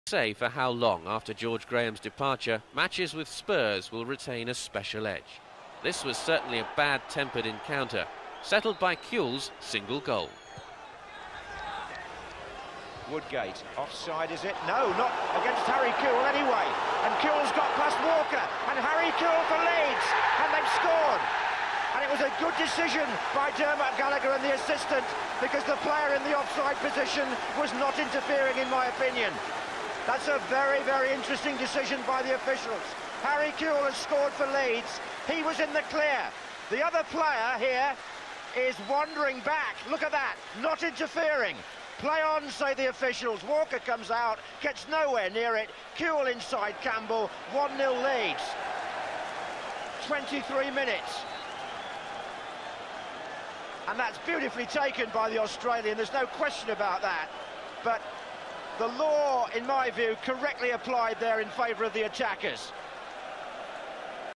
Leeds United movie archive - Leeds v Tottenham Hotspur - 12/02/2000
February  12th 2000 Tottenham Hotspur (H) 1-0 (W) Harry Kewell attendance 40,127
Leeds continue their challenge for the Champions League